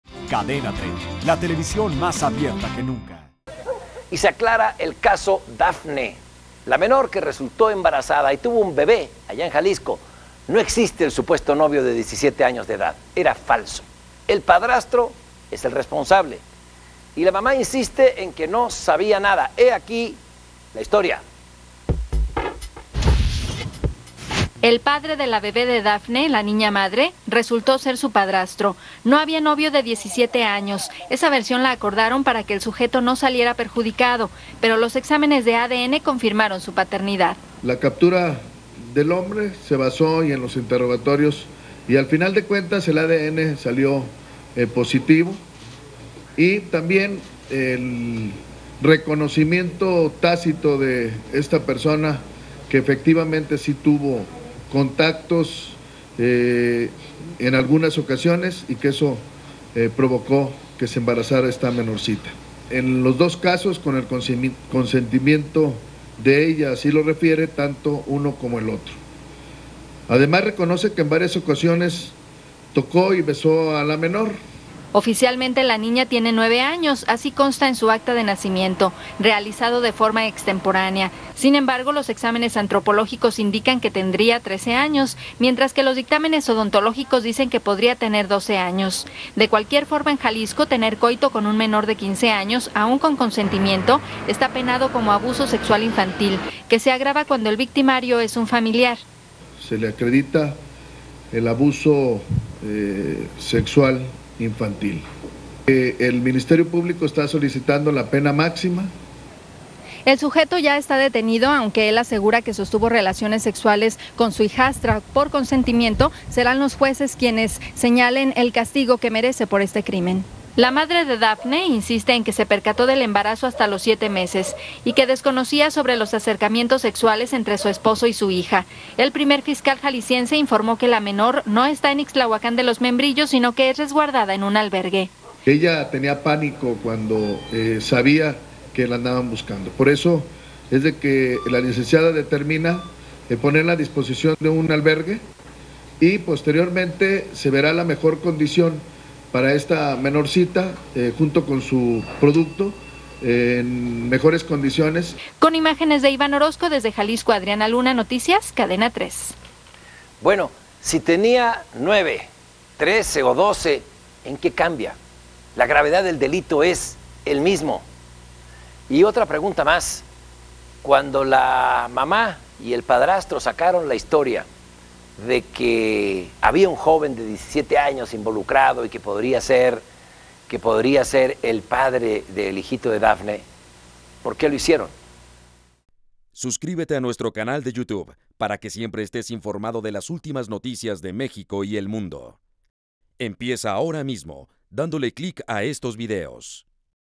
Padrastro embarazó a Dafne, la menor de Jalisco. Cadenatres Noticias
De acuerdo con exámenes de ADN, Abundio García Guadalupe, de 44 años, es el padre biológico de la bebé nacida en el hospital de Zoquipan. Asimismo, el detenido afirmó tuvo relaciones sexuales con la menor con el consentimiento de ella.